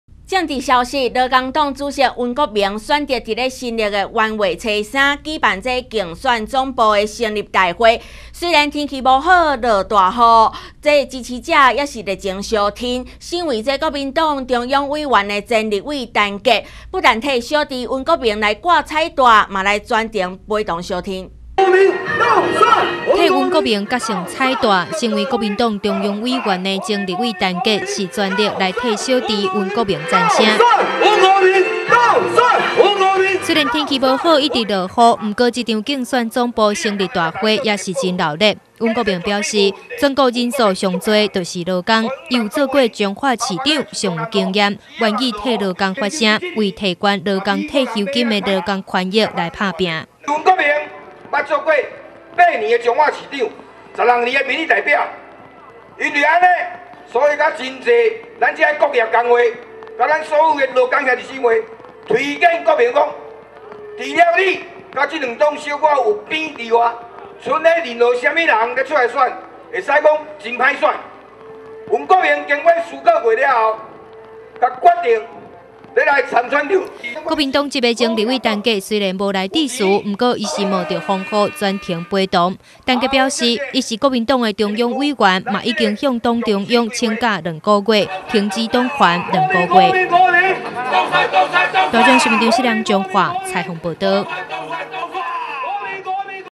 1050105風雨生信心 温國銘競選總部成立
勞工黨主席温國銘選擇在元月3日下午舉辦競選總部成立大會，雖然天候不佳，但大雨澆不熄支持者的熱情，身為國民黨中央委員的前立委陳杰不但替胞弟温國銘披掛彩帶，更是不畏風雨全程陪同